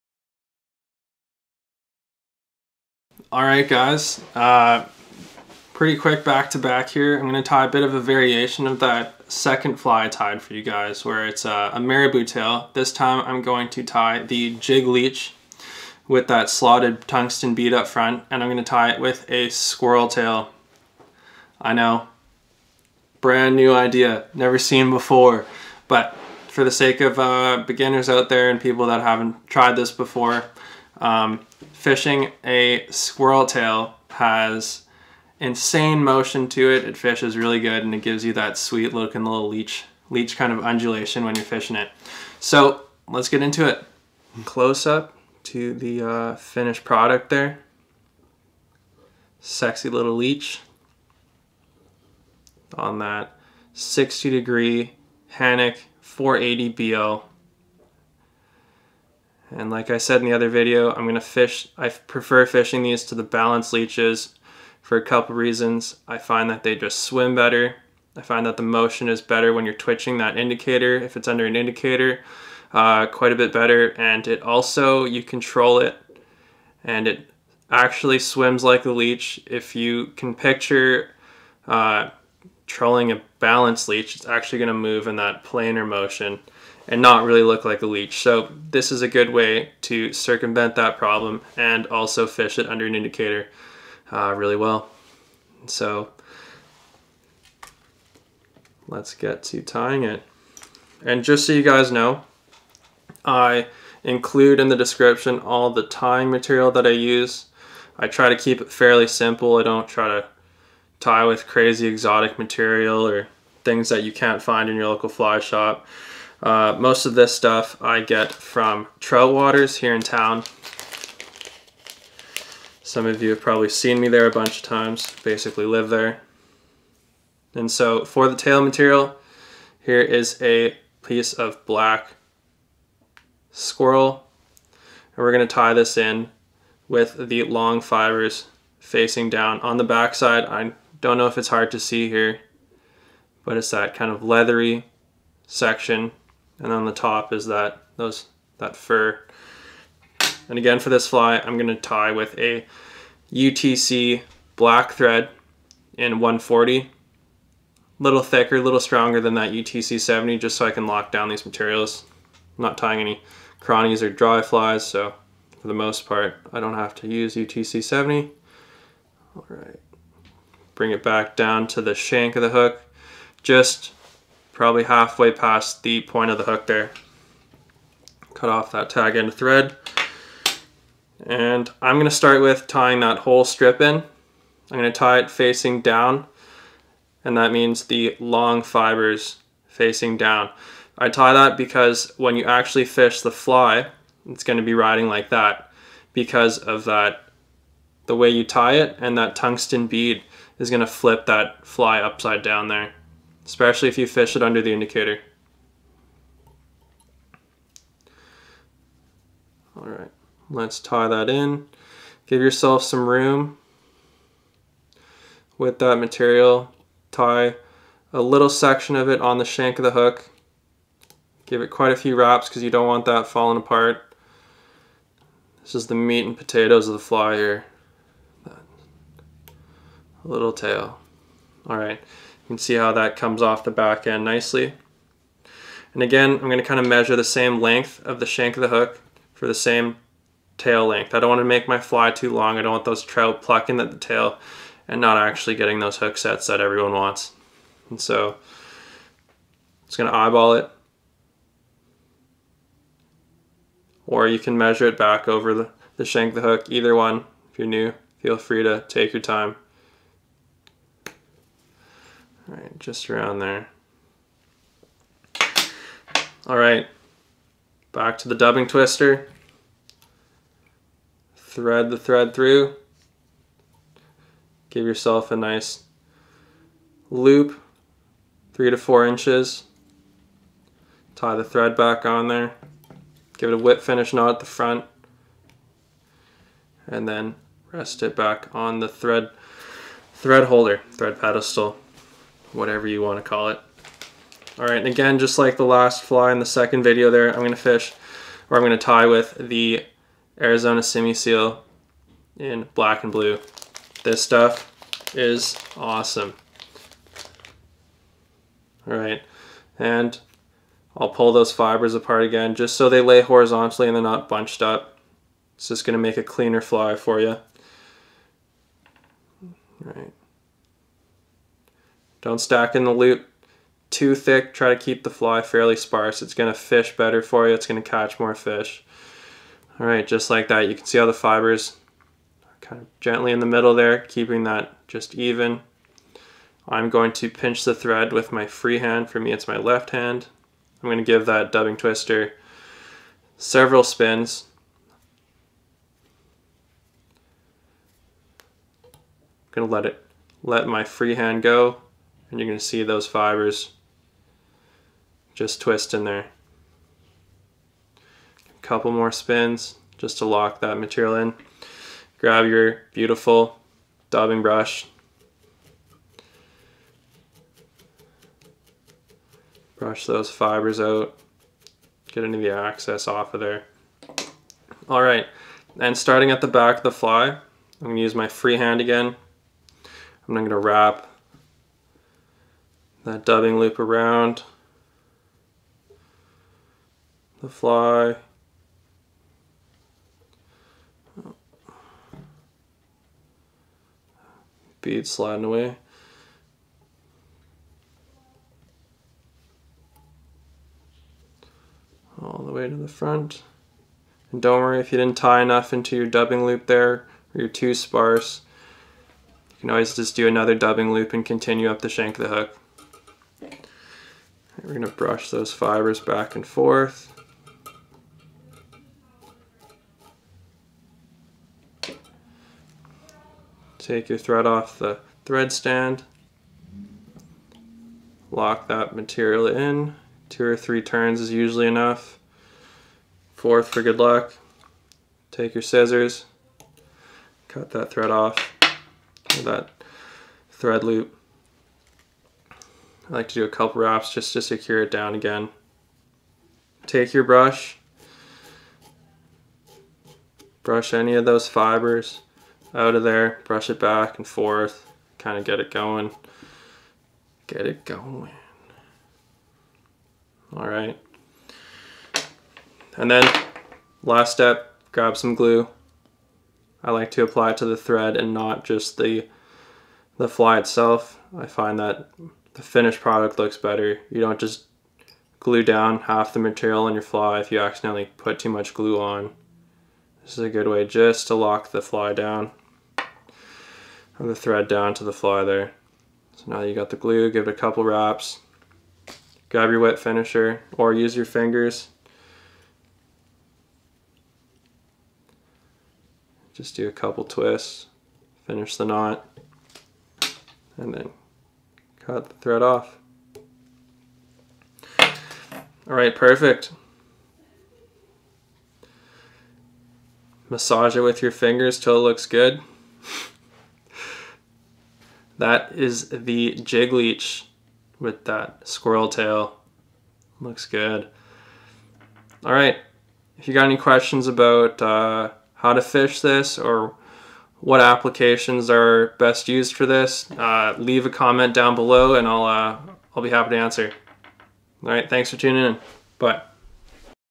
Stillwater Fly Tying: Jig Squirrel Tail Leech
A must have in every stillwater anglers fly box!

Here's the material list:
Hook: Hanak 480BL Size 8-12
Thread: UTC 140 Black
Tail: Black Squirrel
Body: Black/Blue Arizona Simi Seal

This fly works in many different colour variations including red/black, maroon, brown and olive.